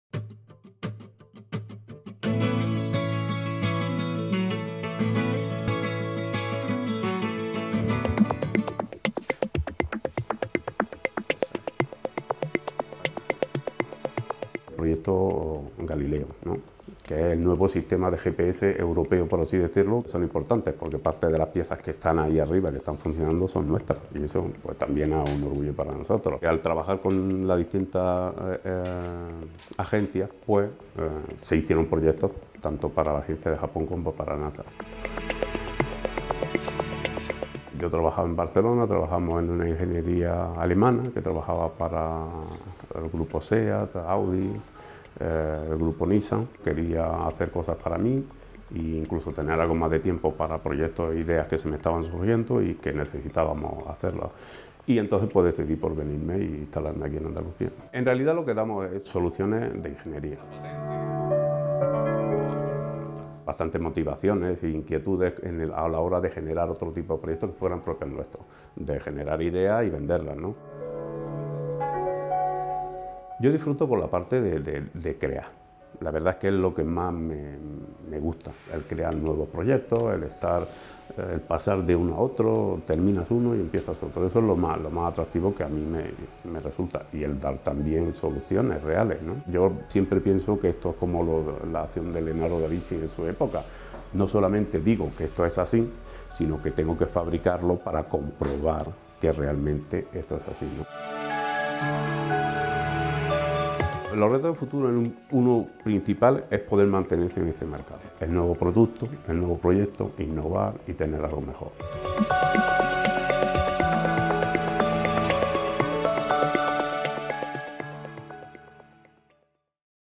HDL Enrique Castillo, soluciones de ingeniería para explorar el espacio
Dejó de trabajar como ingeniero en Barcelona y volvió a su tierra, a Granada. Enrique Castillo es el responsable técnico de una empresa que ha desarrollado algunas piezas para la Agencia Espacial Europea. Han colaborado en el proyecto 'Galileo Galilei', con la Agencia Espacial Japonesa y con el Instituto de Astrofísica de Andalucía. Desde la localidad granadina de Cijuela desarrollan también sus propios prototipos.